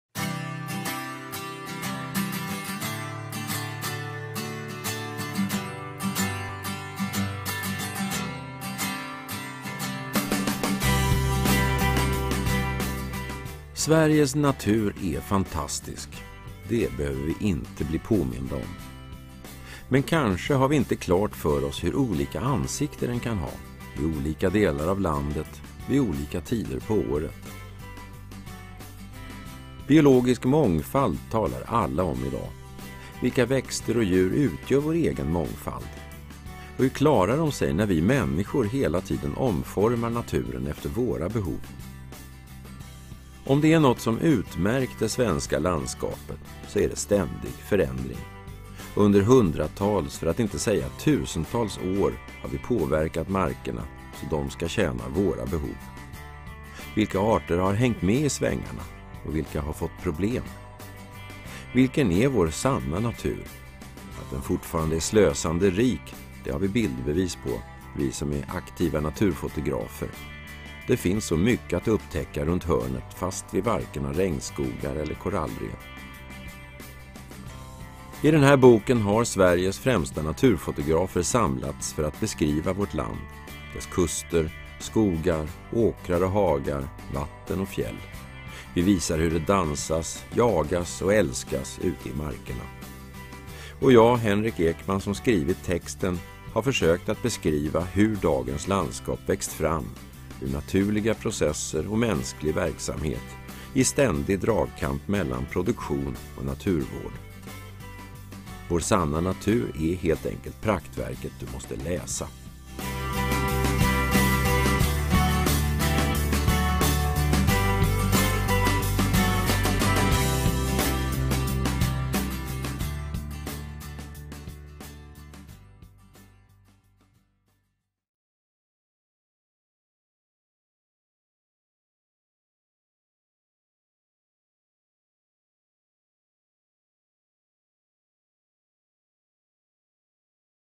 Vår sanna natur -- Naturfotograferna ser på Sverige
Sveriges natur är fantastisk. Men vad består den där naturen av? Vad är det för växter och djur som lever där och som utgör den omtalade mångfalden? Henrik Ekman och Naturfotograferna tar i praktverket Vår sanna natur med sig läsaren på en resa av sällan skådat slag.

Intresset för det levande är större än någonsin, engagemanget starkt. Men vi måste inte drömma oss bort till regnskogar och tropiska rev. Tångruskor och färggranna fiskar, trolska skogar och vilda djur finns även på nära håll, bara vi öppnar ögonen.